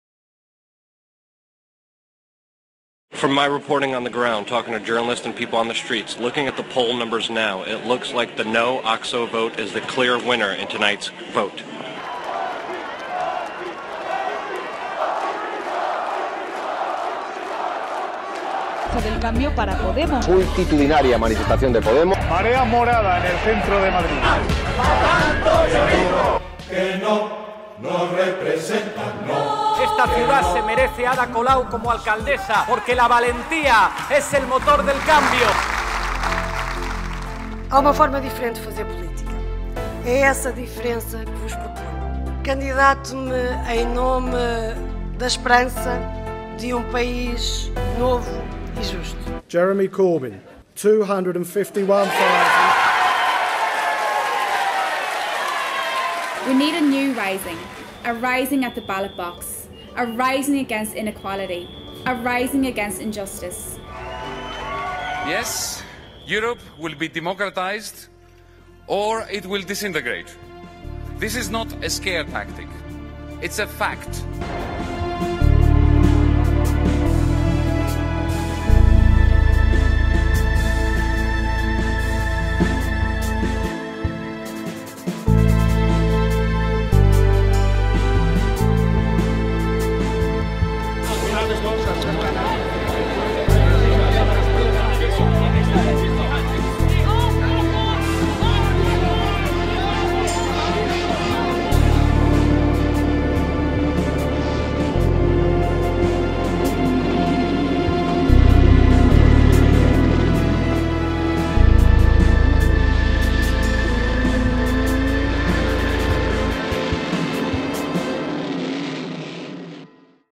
Intro Democracy in Europe | Democrazia in Europa 23Mar2016
Intro video "Democracy in Europe | Democrazia in Europa "
w. Marisa Matias, Jeremy Corbyn, Yanis Varoufakis, Pablo Iglesias
23M 2016 | Roma